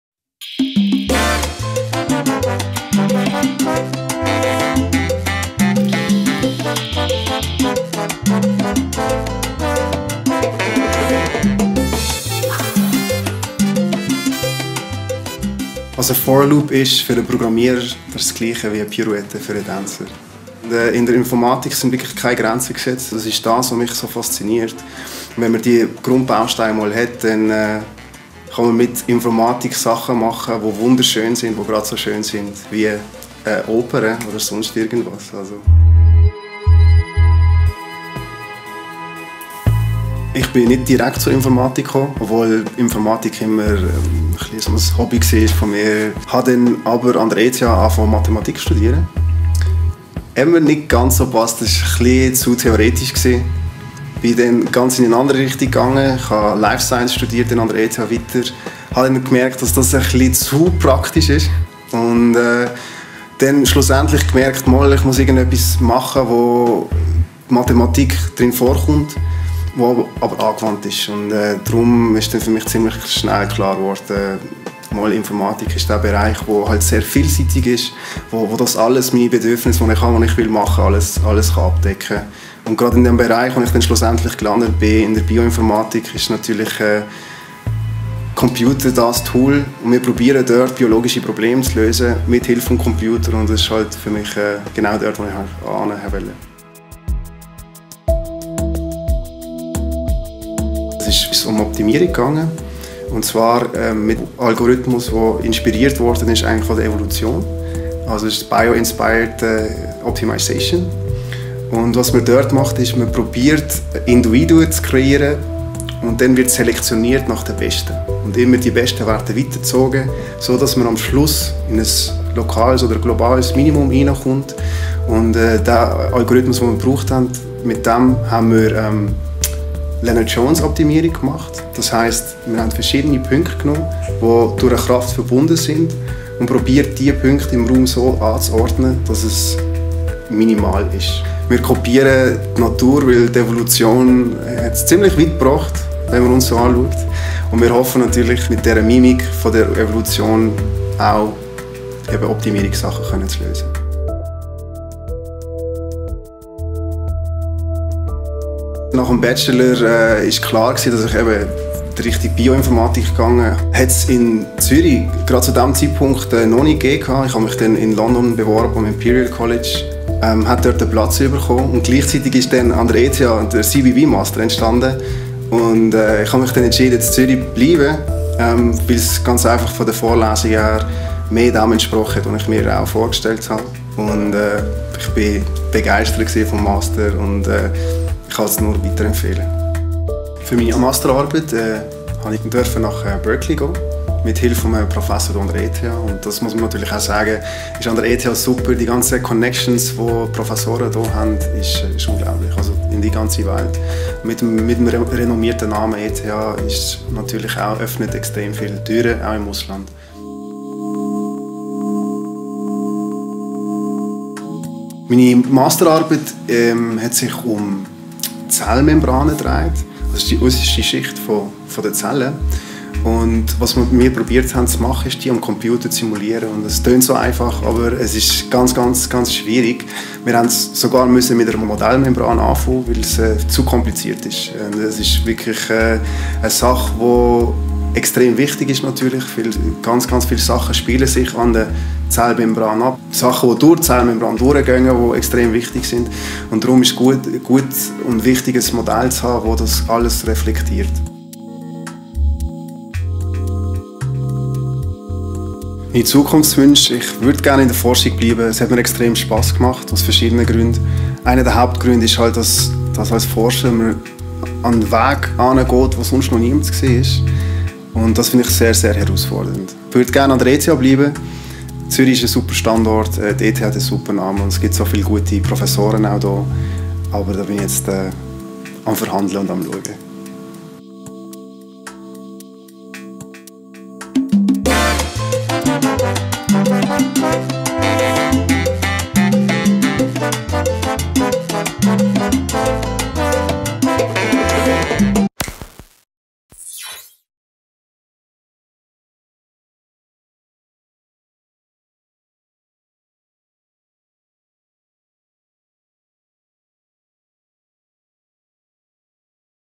Yannick - ETH Zurich - Department of Computer Science
(Language: Swiss German)
Yannick was among the first students who finished their master studies in computational biology and bioinformatics at ETH Zurich. In his video he tells how he made his way from mathematics to life science and why he finallly ended with computer science.

In between, Yannick successfully finished his master studies and changed to Universitaet Zurich where he started his doctoral studies in Physical Chemistry. The science of physical chemistry is the fascinating combination of two basic natural sciences. With the help of physical chemistry new methods to analyse the behaviour of molecules in their environment or to analyse molecule structures can be developed. The research for super hydrophobic surfaces or the development of very thin, biologically active membranes are further fields of research. People with different backgrounds are working at the institute, which proofs the well known interdisciplinarity.

Yannick is a very passionate and talented salsa dancer - watch the video and get the proof! And if you want to see him dancing Zouk with Anna, our doctoral student from Poland, check out this video

Yannick: „Mein Weg in die Informatik war eine kleine Odyssee. Ich hatte immer schon Spass an Mathe, und so entschied ich mich nach der Matur für das Mathematikstudium. Bald merkte ich, dass mir diese rein theoretische Welt doch nicht richtig lag und wechselte zur handfesten Biowissenschaft (life science). Aber dann entdeckte ich die Informatik und mit ihr die Möglichkeit, die Theorien der Mathematik zu praktischen Werkzeugen um zubauen und damit in allen möglichen Bereichen an vorderster Front aktuelle Probleme lösen zu können. Ich machte meinen Bachelor in Informatik und fand anschliessend im spezialisierten Masterstudiengang Computational Biology and Bioinformatics die für mich perfekte Kombination aus Theorie, Informatik und Biowissenschaften. Erst durch die anfänglichen Umwege wurde mir bewusst, wie wichtig mir diese Interdisziplinarität und Wandelbarkeit tatsächlich ist.

Eine weitere grosse Leidenschaft ist für mich das Tanzen. Im Tanz geht es darum, einen präzisen Fluss zu schaffen, in dem man sich verlieren kann, wo sich Fantasien verwirklichen lassen. Auf etwas andere Art geschieht dies auch beim Programmieren. Beide Disziplinen, der Tanz wie auch die Informatik, erfordern vor allem zu Beginn viel harte Arbeit, und das Drehen der ersten Pirouette beim Tanzen gleicht der plumpen „for loop" beim Programmieren.

Das erste Studienjahr ist recht hart. Wenn diese Hürde aber bezwungen ist, geht es danach richtig kreativ los: man kann anfangen zu spielen und zu konstruieren mit den Fähigkeiten, die man sich angeeignet hat, und mehr und mehr werden eigene gute Ideen zum eigentlichen festen Boden. Wie ein Choreograph fängt man an, eine dynamische Vorstellung zu entwickeln, verschiedene Ansätze zu finden, zu probieren, um oft auch wieder zu verwerfen und neu zu beginnen -- auch das gehört dazu und ist enorm wichtig und spannend.

Ich liebe Informatik, weil sie kreativ, innovativ und herausfordernd ist - man kann in ihr eigentlich alles finden, was man sich vorstellt. Die Informatik ist ein wahres Chamäleon und entspricht damit meinem Leben und meiner Persönlichkeit."

Nach erfolgreichem Abschluss seines Masterstudiums hat sich Yannick für ein Doktorat an der Universität Zürich entschieden und arbeitet für das Institut für Physikalische Chemie.